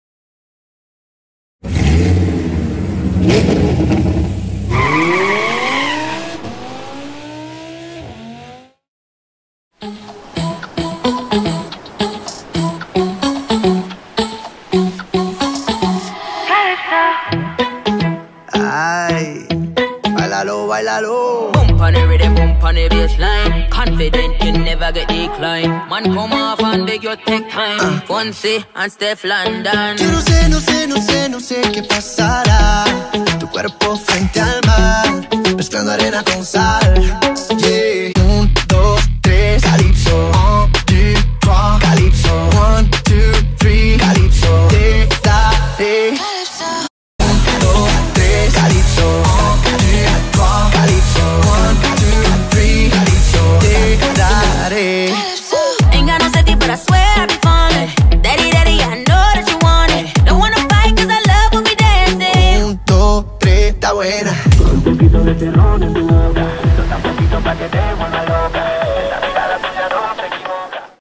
AQPtuning Summer Break 2019 - Punta de Bombon
La Municipalidad Distrital de Punta de Bombon, presenta por tercer año consecutivo el AQPTUNING SUMMER BREAK 2019 - PUNTA DE BOMBON este 16 de febrero desde las 11:30 a.m. en PLAYA 2 - PUNTA DE BOMBON - Arequipa - PERU